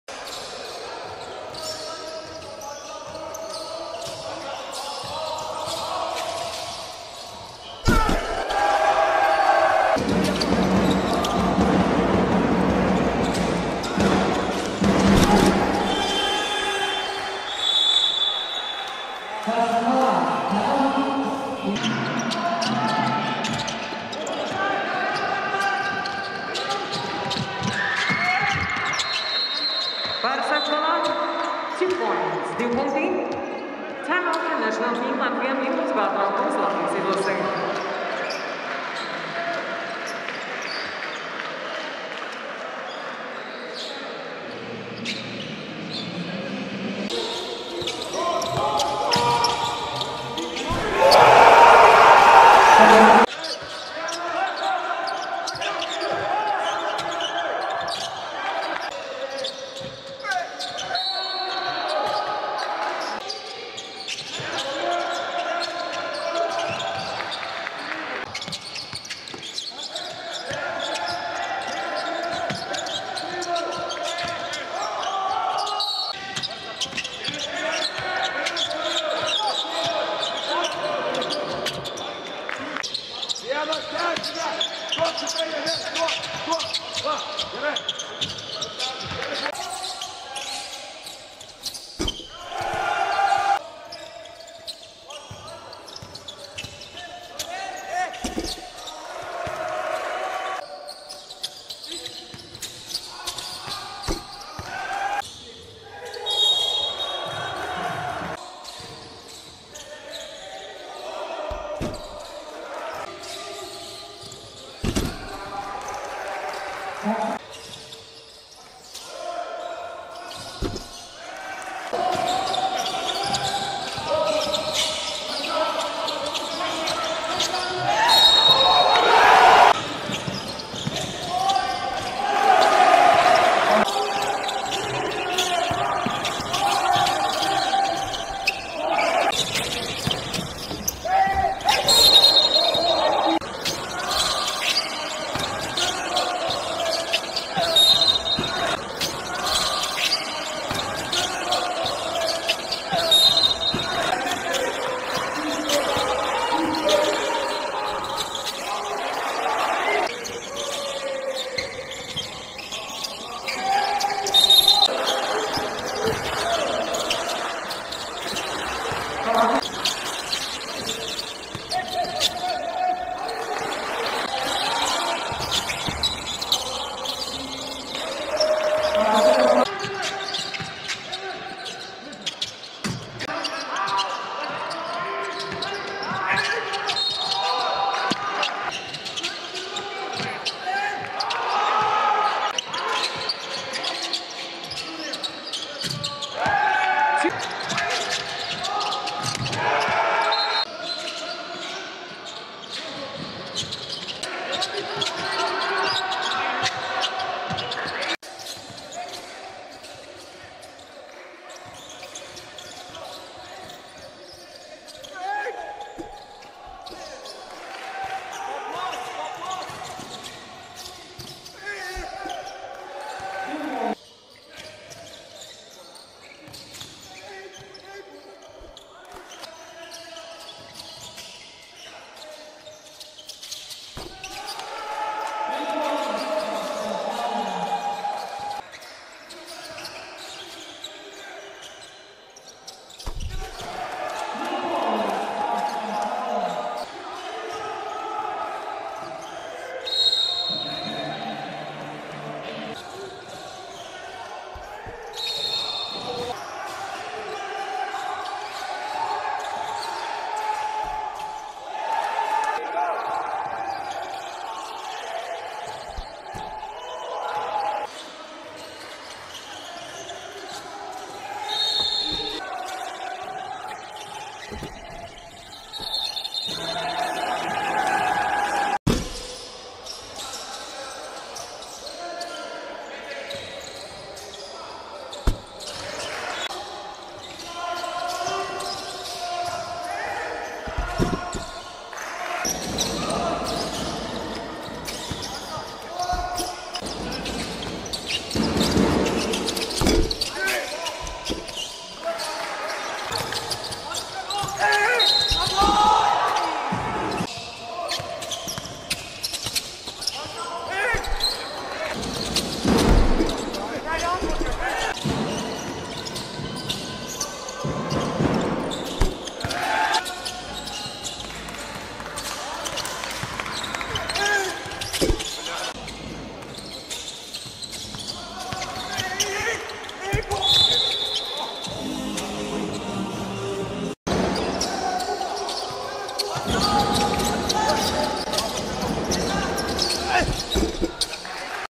Parsa fallah (world cup fiba u19 ) highlight
Top 10 players in world cup

6th player in rebound

10th player in point

9th player efficiency